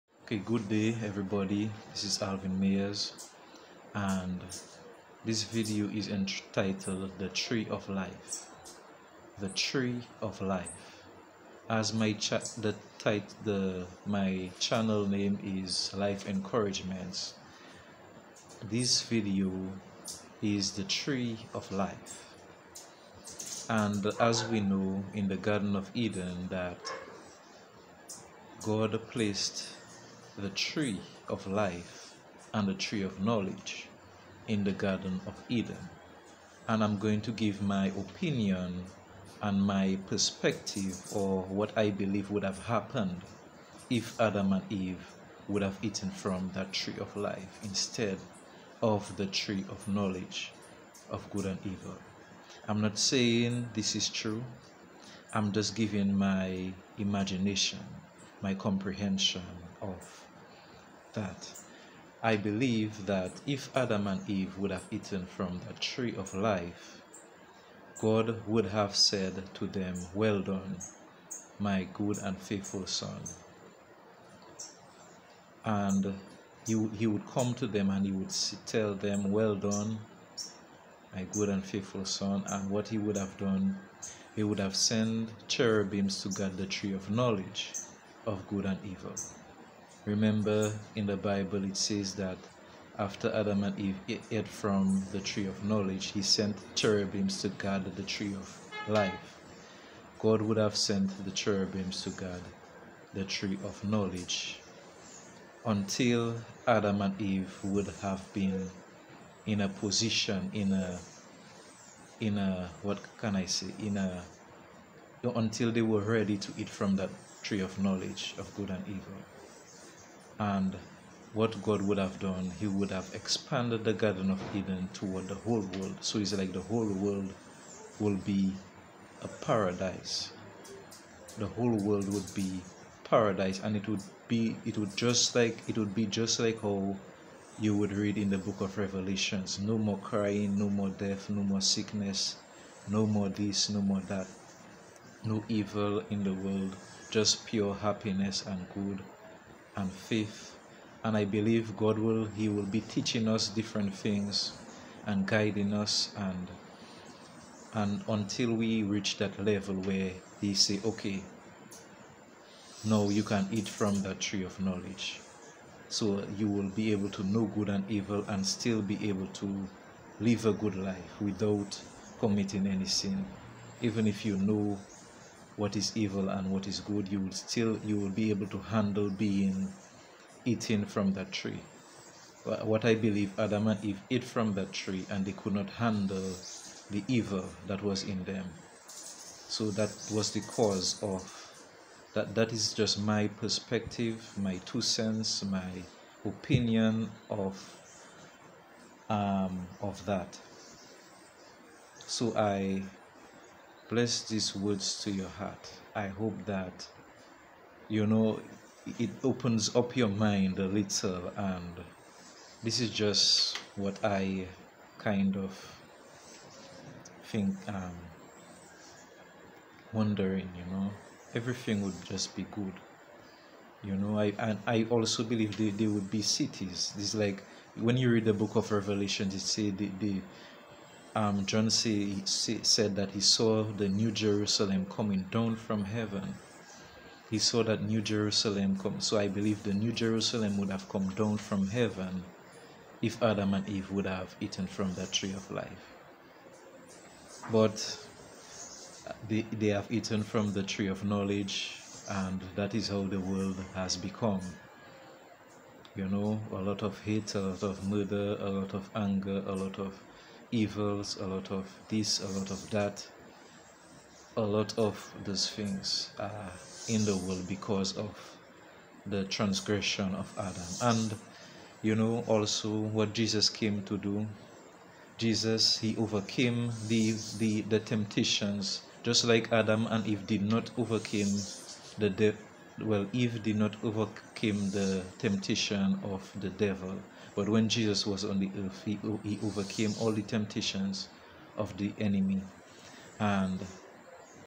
The Tree🌲 of life (MUST Watch)
This is my perspective of what if Adam and Eve had eaten from the tree of Life instead of the tree of knowledge of Good and Evil.